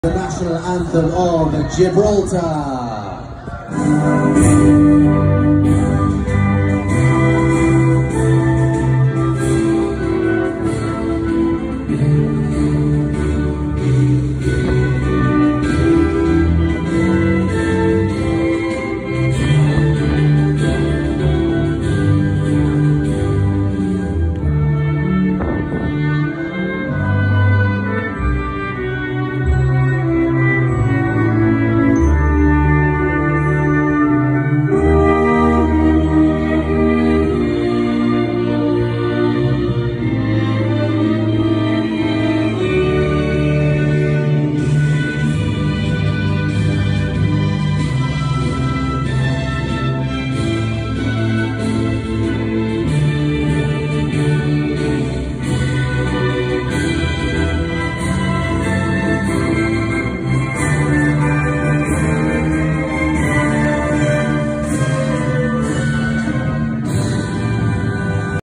Gibraltar vs Latvia (Qualifying round WC 2022)
16/11/2021
Victoria stadium, Gibraltar